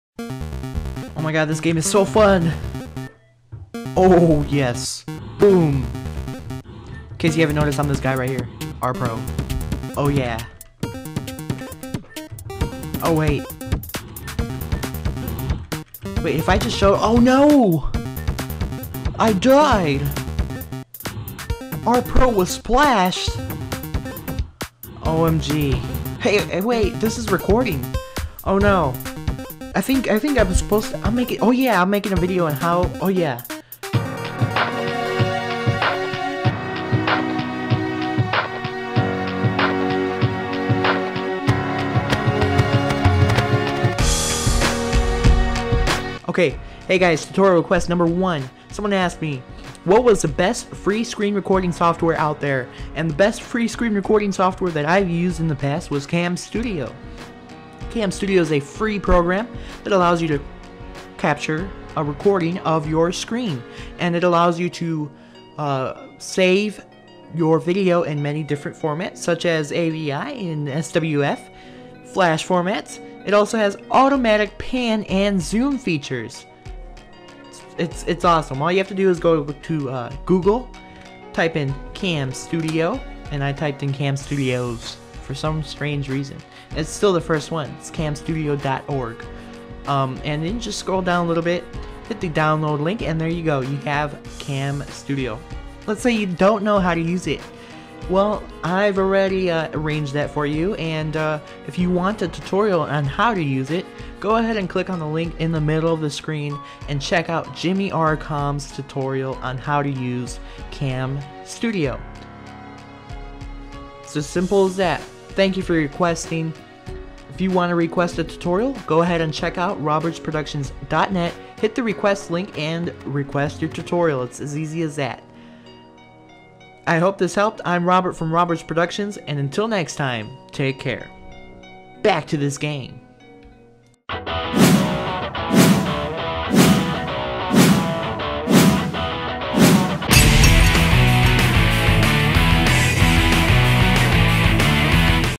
Request #1: A Decent FREE Screen Recorder
"parkour134" has requested the first tutorial on the new He wanted to know where he could find some FREE Screen Recorder Software" out there. Here is the tutorial on what I think is a decent screen capturing software that's absolutely free. It's Go ahead! Check it out! Thanks for requesting! Remember! Keep those tutorial requests coming!

Please Note: I was not using Camstudio in the making of this video. I was just demonstrating a free piece of software people could use for screen capture. This was a requested tutorial.

Created By: Robert's Productions Studios. Now In HD.